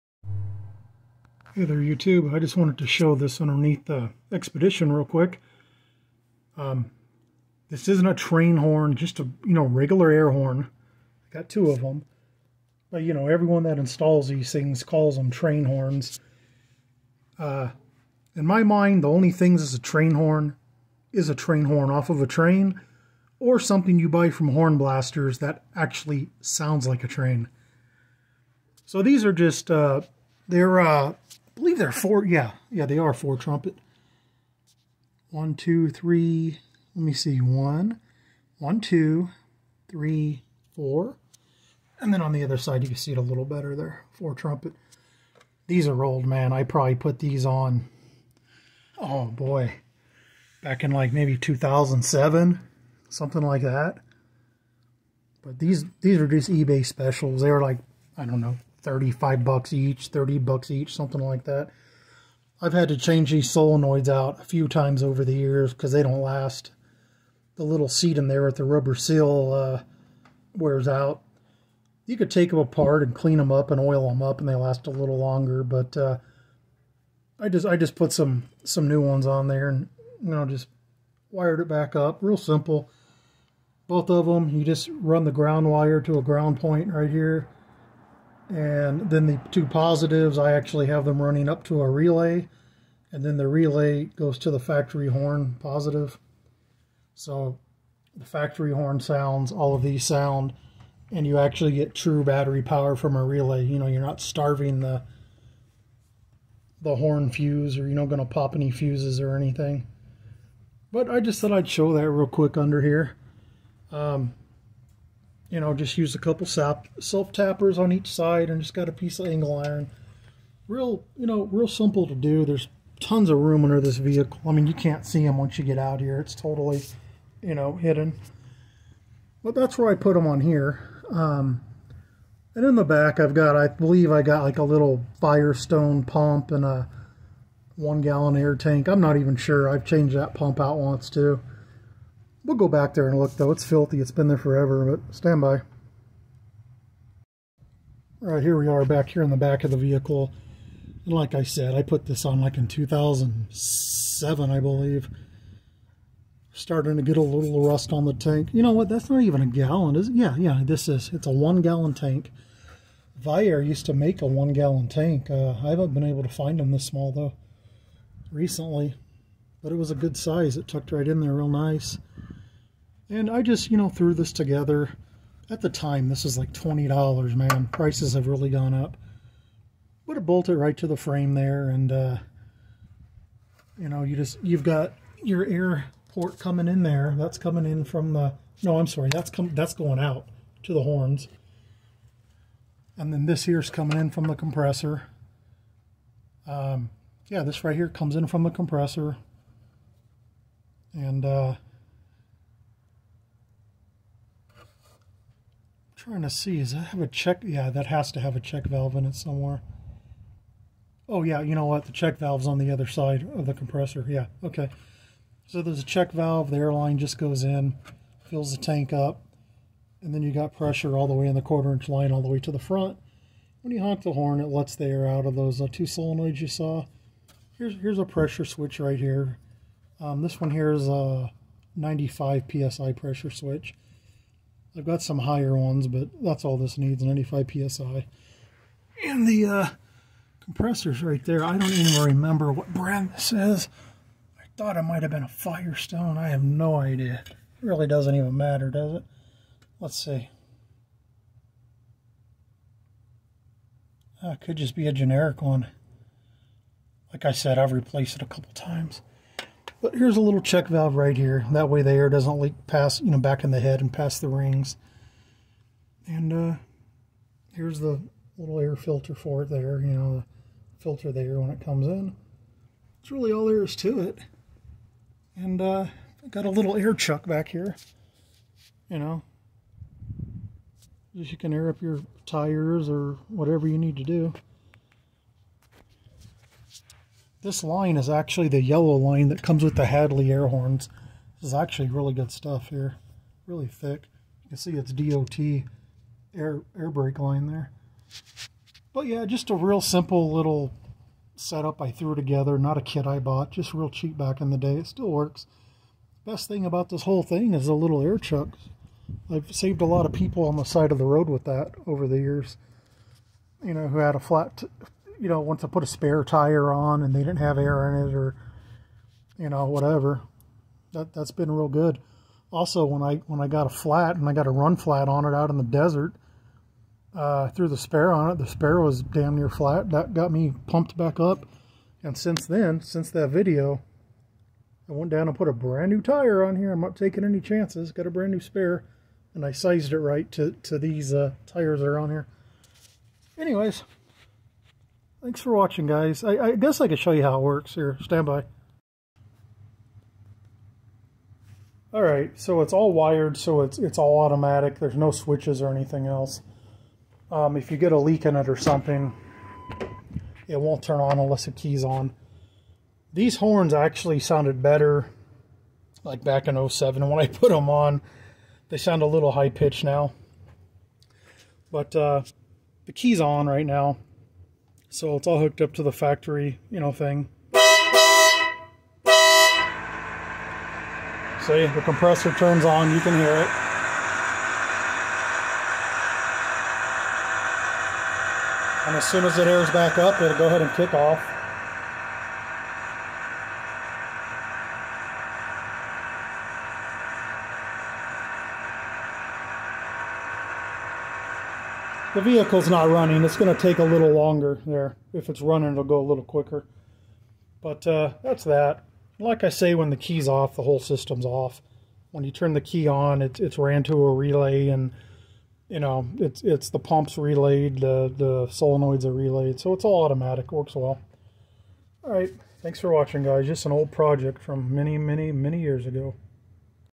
Air horn installation
I installed this back in 2007, just a very simple install of gathered up parts. Totally automatic, properly relayed and fused.